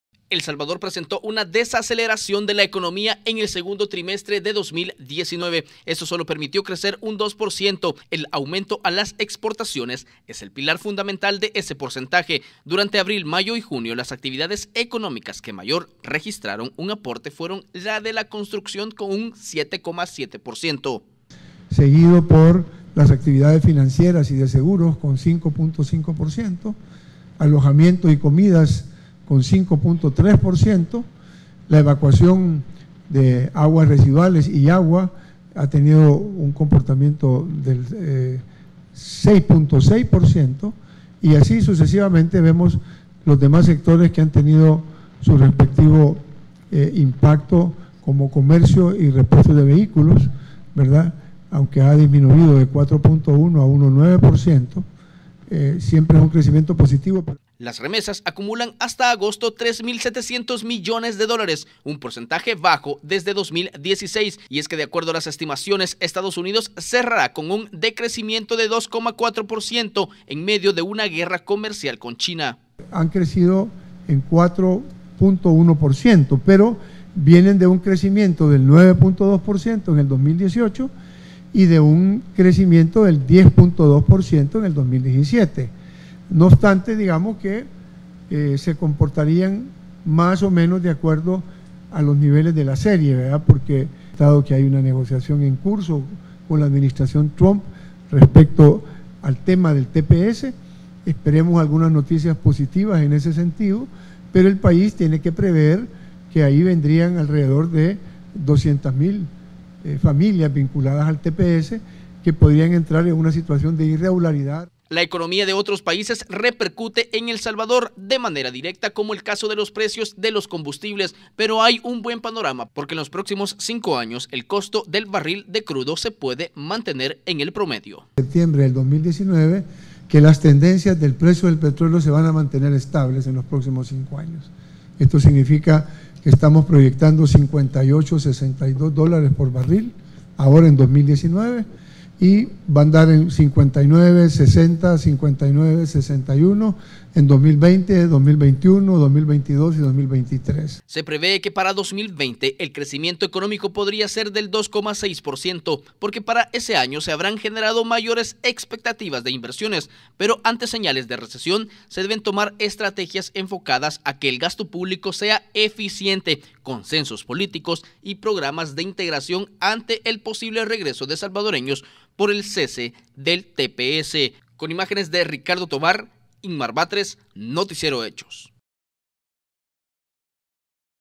Perspectiva económica BCR II trimestre
El Salvador presento una desaceleración económica en el segundo trimestre del año